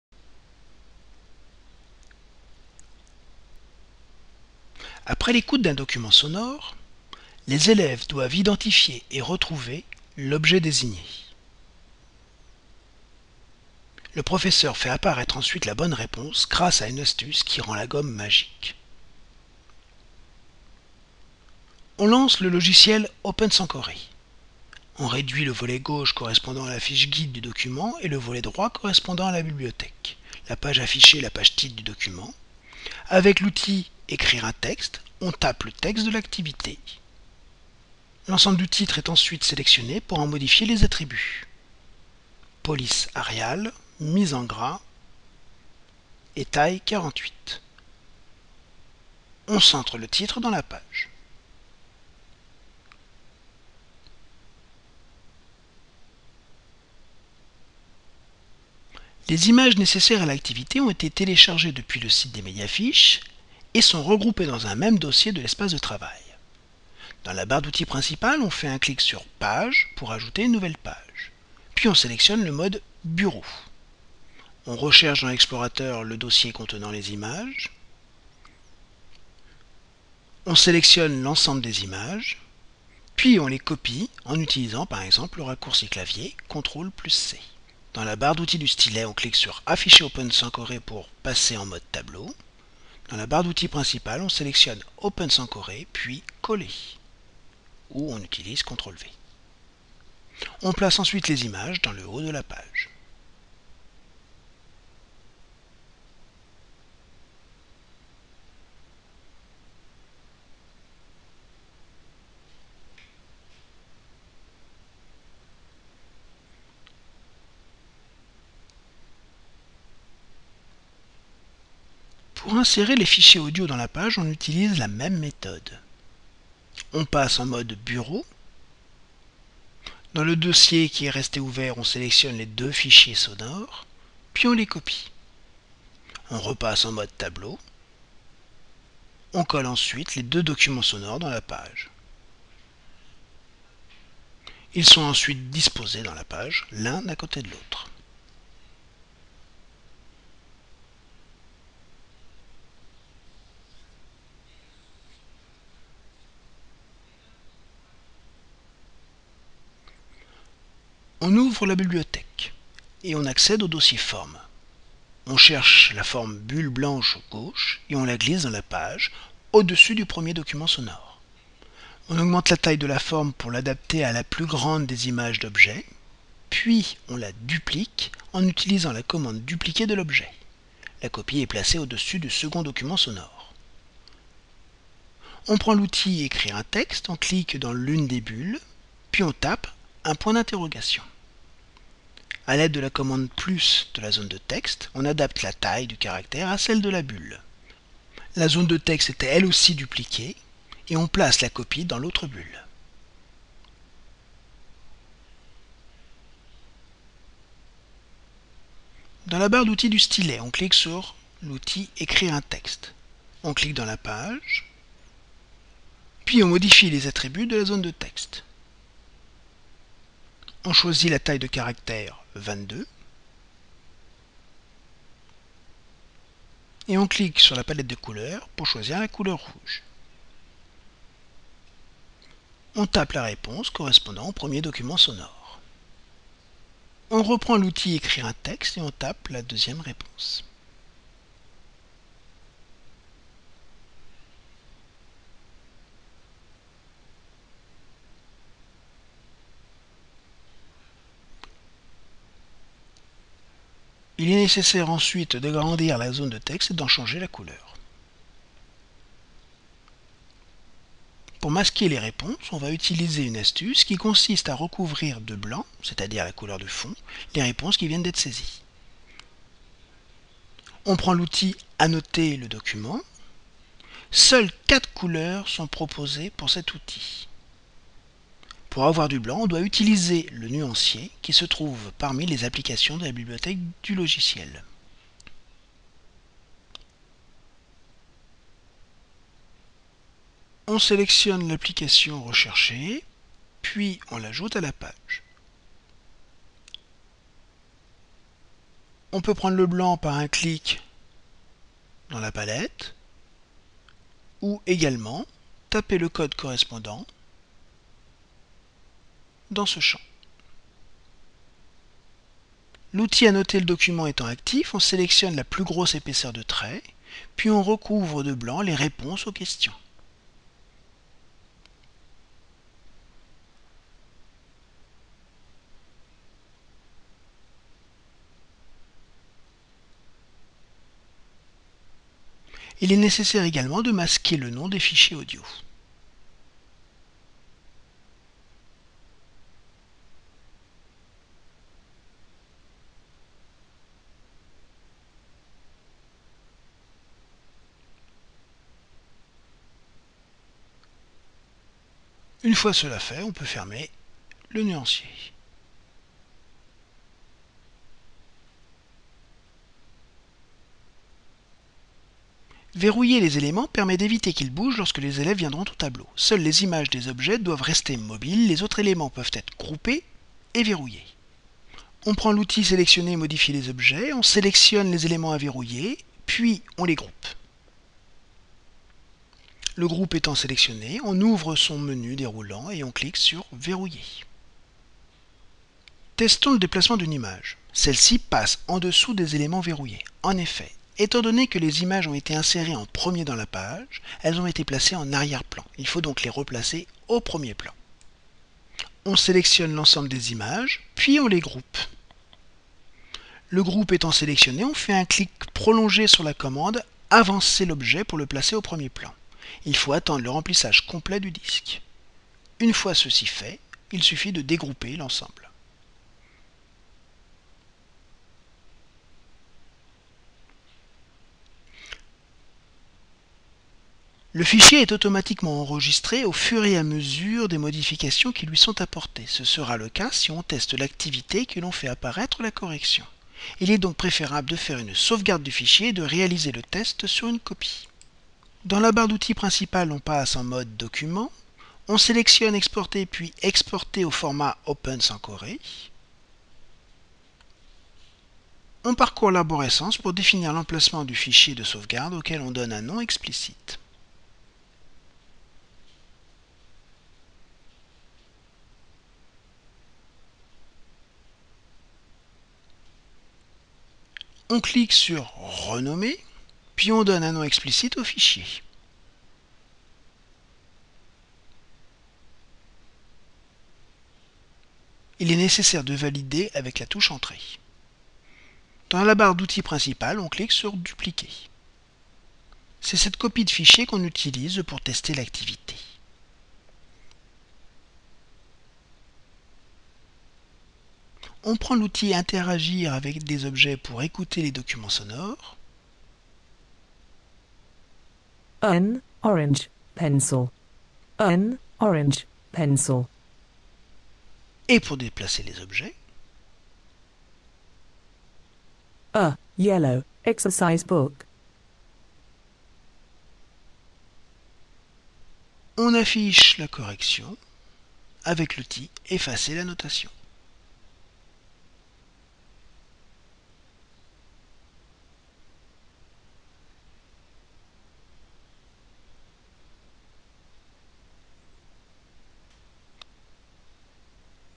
Fiche 8 - Tools for students - Open-Sankoré
Les élèves écoutent un document sonore en anglais, qui désigne un des objets affichés sur la page. Un élève vient au tableau déplacer l'objet qu'il a identifié sur l'emplacement prévu. Le professeur fait, ensuite, apparaître les écritures correspondantes grâce à l'utilisation de la « Gomme magique ».